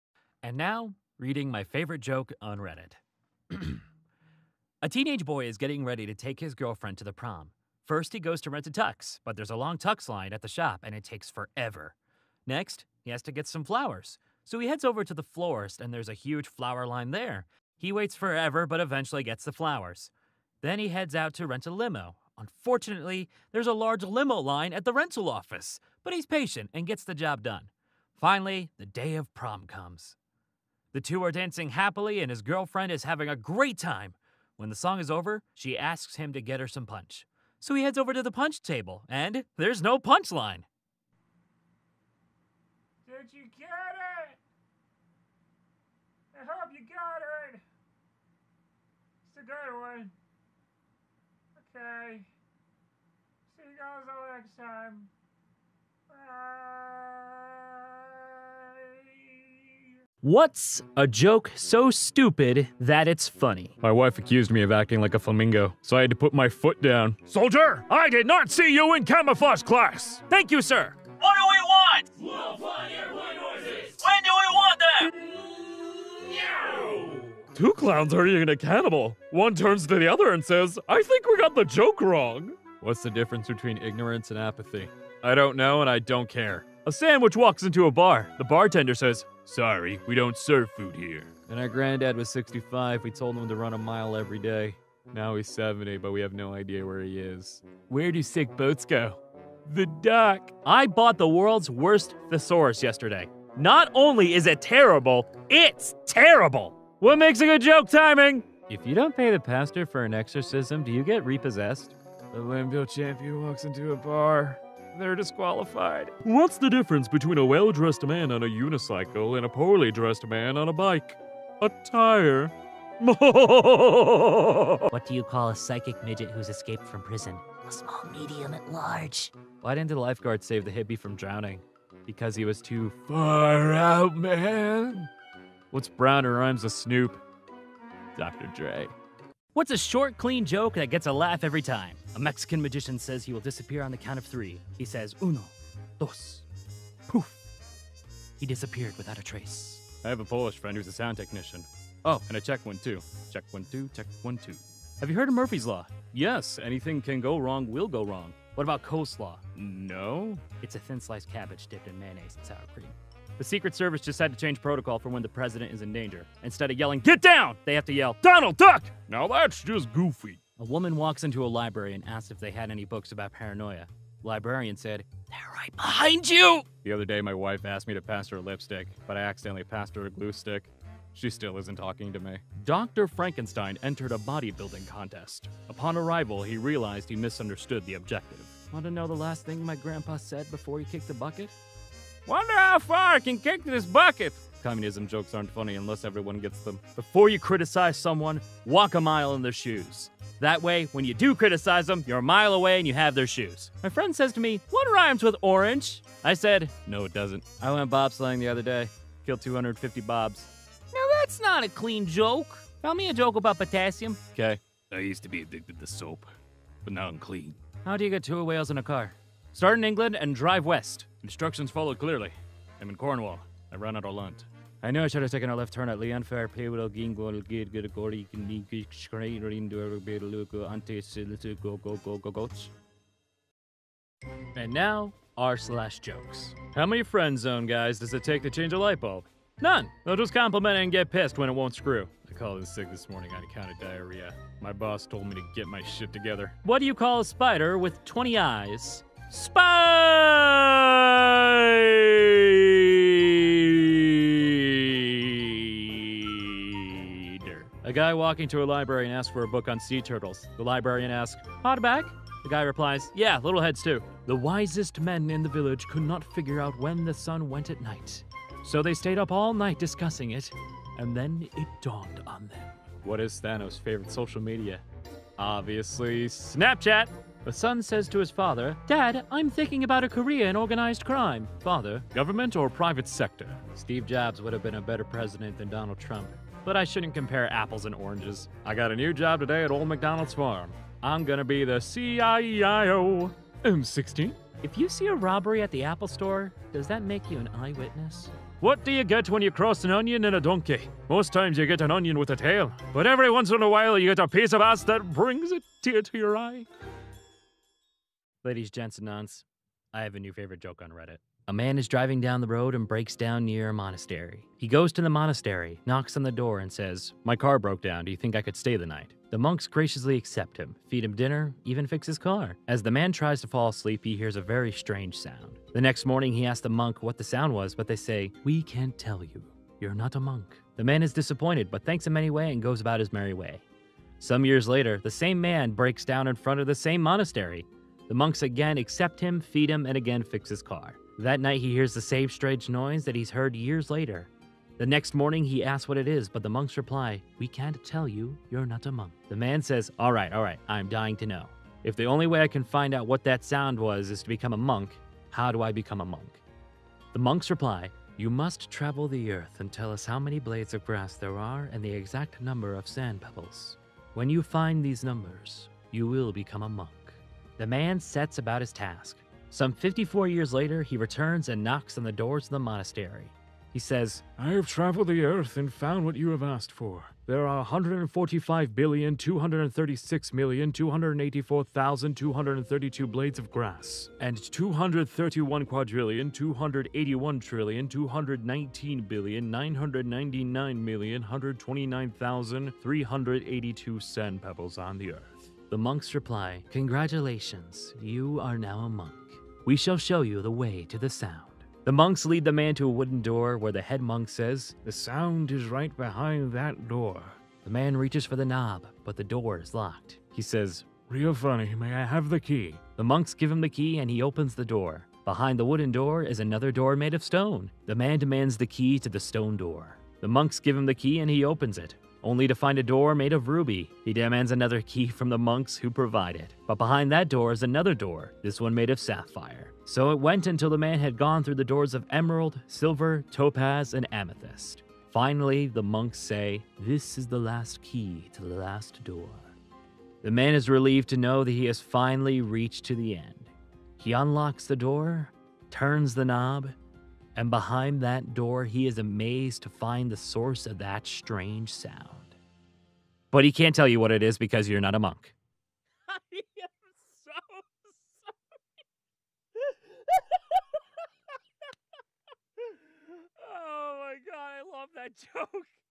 Jokes From Reddit [Binge Compilation]
Comp of all the jokes done from Reddit.
Even that one.